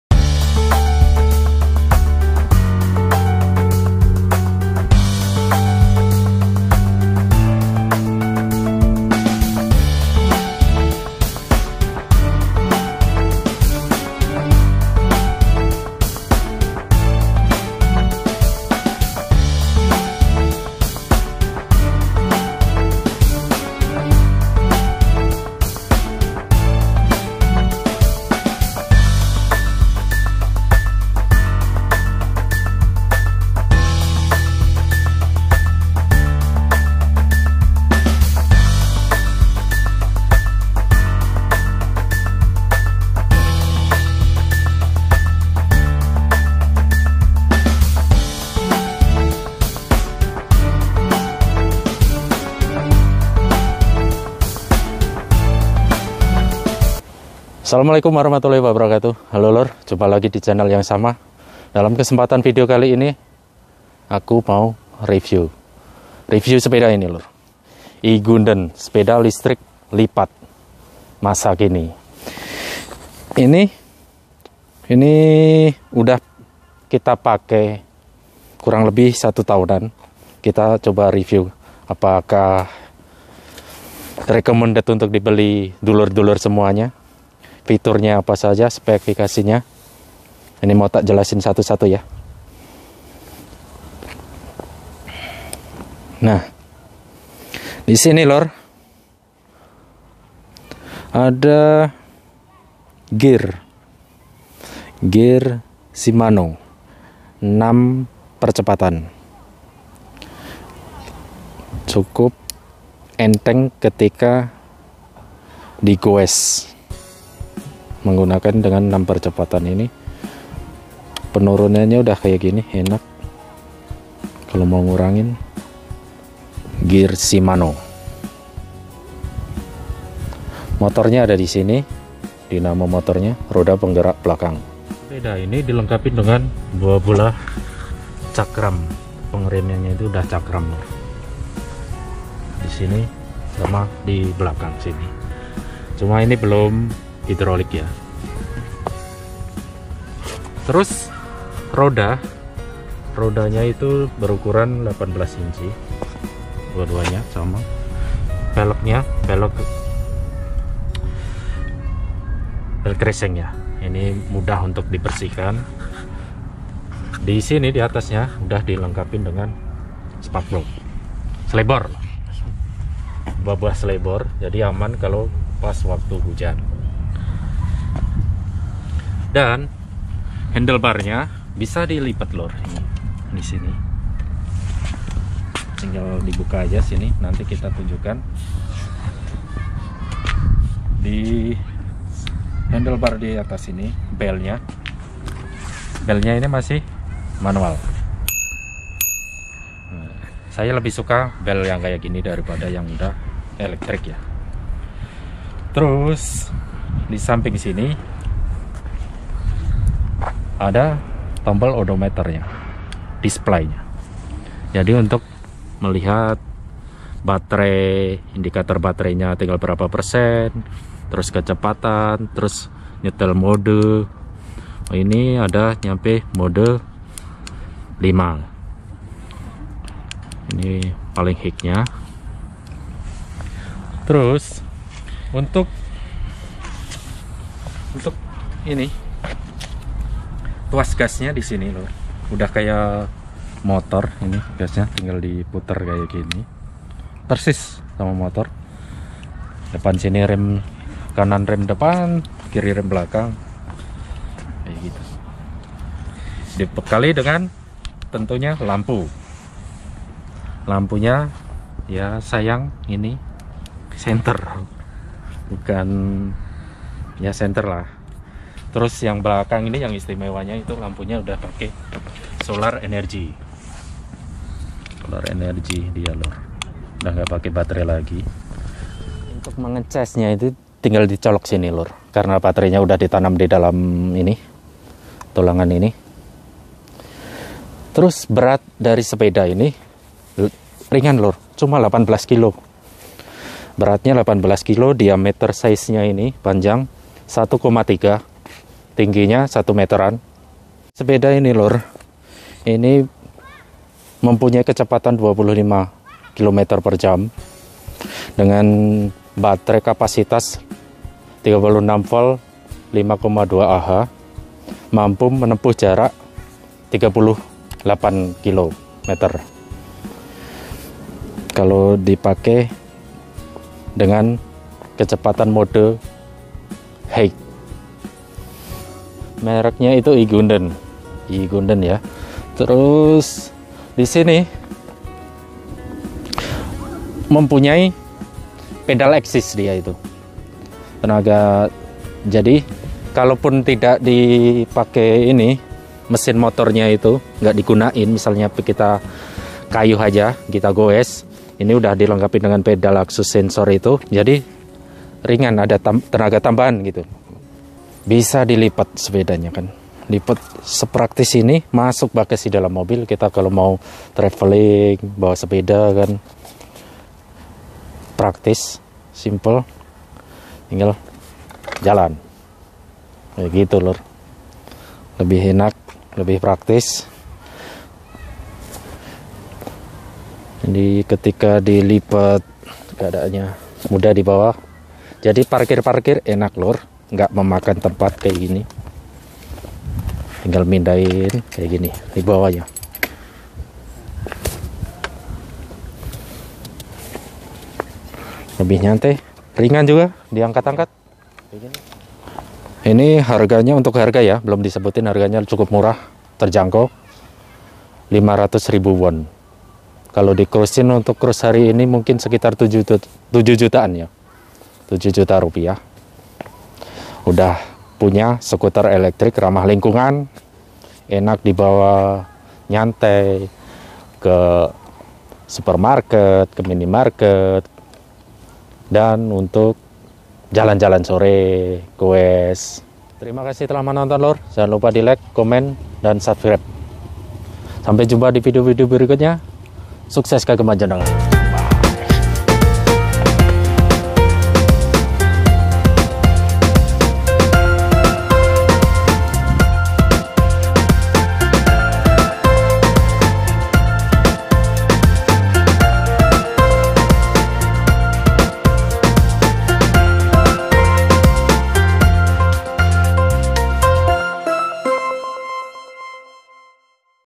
Riview Sepeda Listrik Lipat Bukan Kaleng-kaleng‼️
sepeda listrik lipat merk egeunduun adalah sepeda listrik lipat termurah di kelas nya.
ini adalah review sepedaListrik ramah Lingkungan sekaligus ramah di kantong.